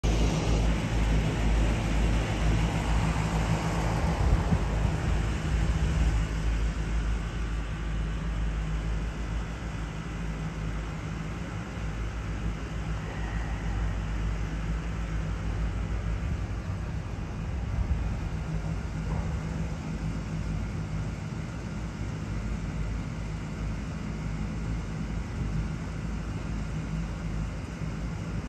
Bird sits on a parking lot streetlight
Got a bird sitting on a streetlight on a sunny afternoon.